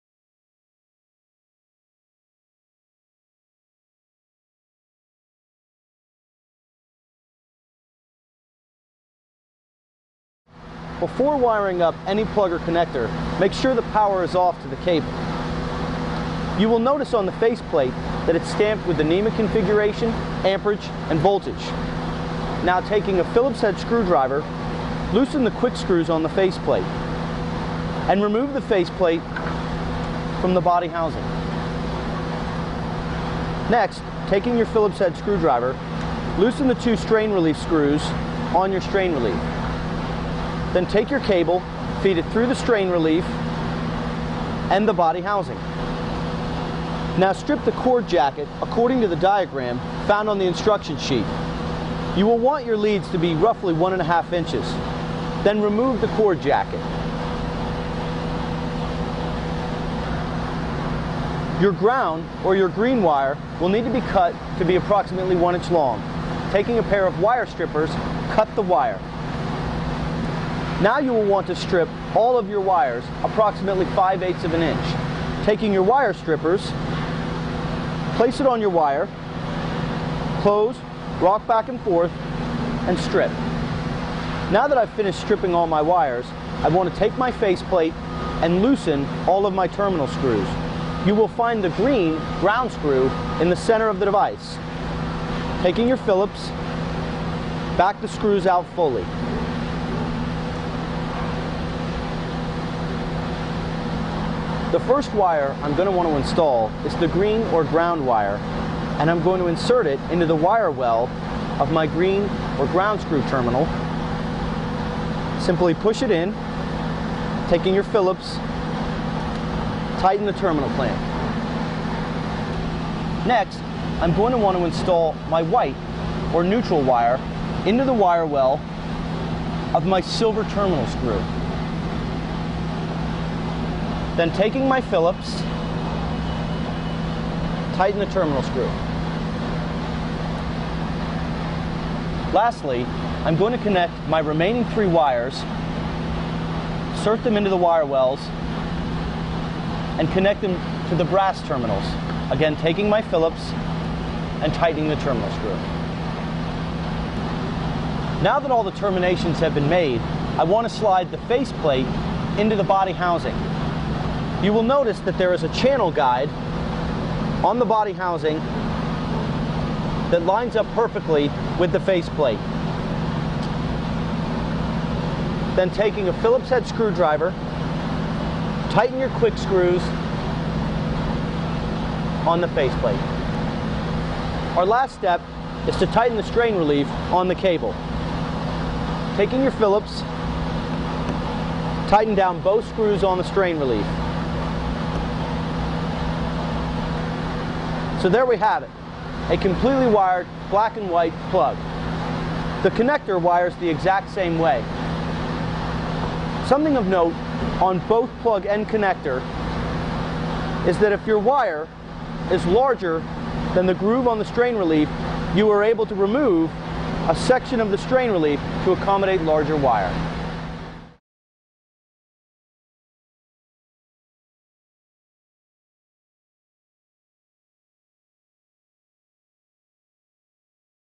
How to install a Leviton Industrial Locking Wiring Device
Leviton's industry leading locking devices, such as our Black & White line of industrial grade plugs and connectors and receptacles, offer superior performance, long lasting reliability and other features that are backed by a lifetime warranty. 20 Amp, 125/250 Volt, NEMA L14-20P, 3P, 4W, Locking Plug, These industrial grade, grounding devices have a super tough nylon body that resists damage from severe Impact, abrasion and chemicals. Solid brass blades and contacts feature improved conductivity, low resistance, low heat, and long life. Internal wiring chambers isolate individual conductors to prevent flashover, arcing, and stray strands of wire.